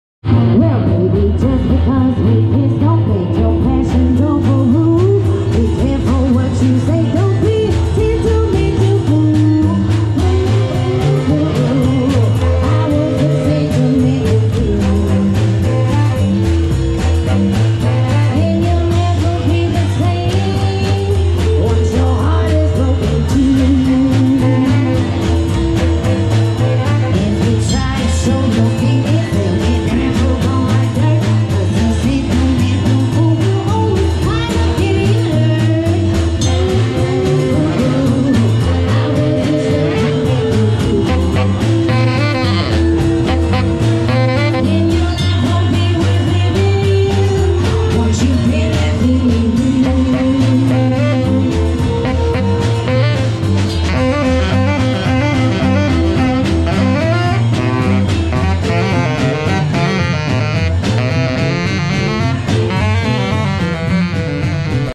Frankie Lee and The Infernos
Frankie Lee and The Infernos playing a birthday party in Las Vegas NV.